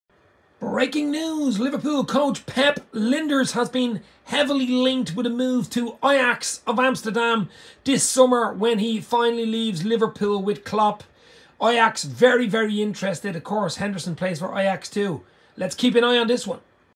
Pep Lijnders linked with Ajax mangers job
Pep Lijnders linked with managers job at Ajax